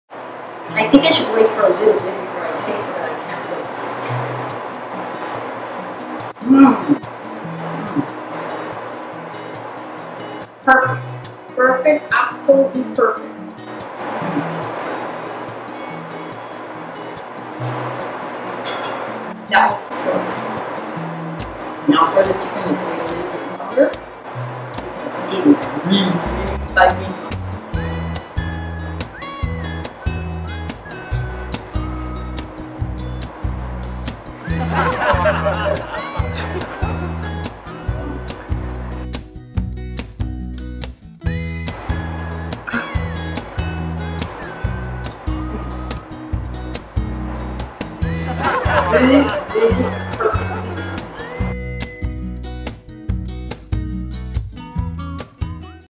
Chef Pachi | FUNNY COOKING BLOOPERS | Wait! It's Hot!
Delicious Diet are Sustainable Diet, with Chef Pachi!
Chef Pachi shares light, fun booking bloopers while filming the shows. Chef Pachi know's the dish is hot but can't wait...so just imagine what happened. Have fun cooking and always be careful.